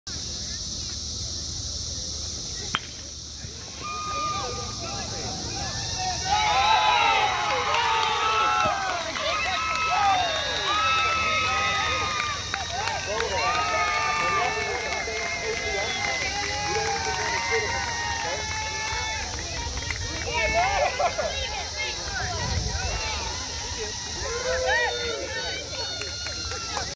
Tyler Goes Yard
Watch Tyler hit a go ahead home run in the ULLL NL Championship game at Mountainview Elementary.